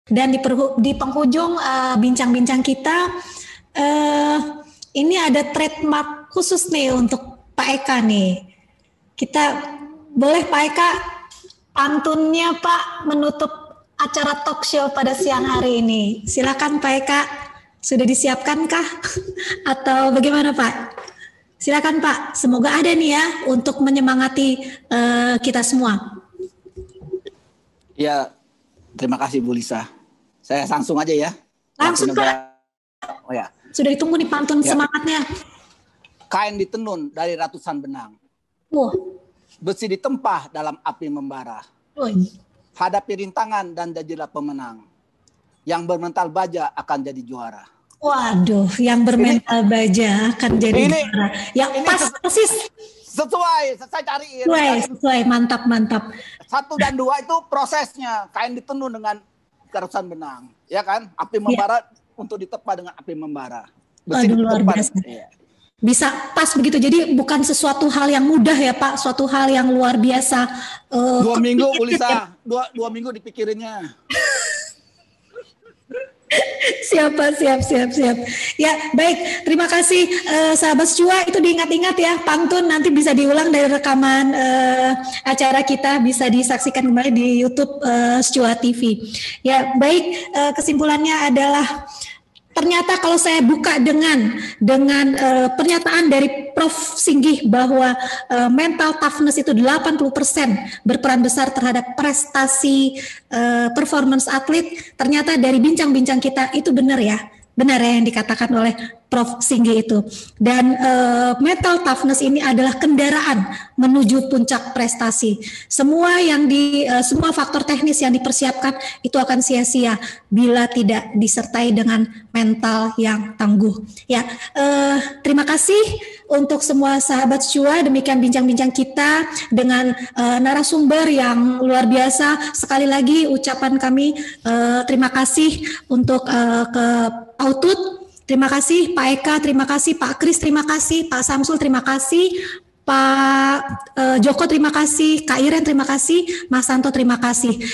Highlight - SCUA Online Talkshow
SCUA Online Talkshow "Mental Toughness For Champions"
Narasumber :
Moderator : WIM Lisa Karlina Lumongdong
Host : WIM Chelsie Monica Ignesias Sihite

Official YouTube Account Sekolah Catur Utut Adianto.

Head Office :
Sekolah Catur Utut Adianto – Bekasi
Jl. Raya Siliwangi no. 15
Rawa Panjang – Bekasi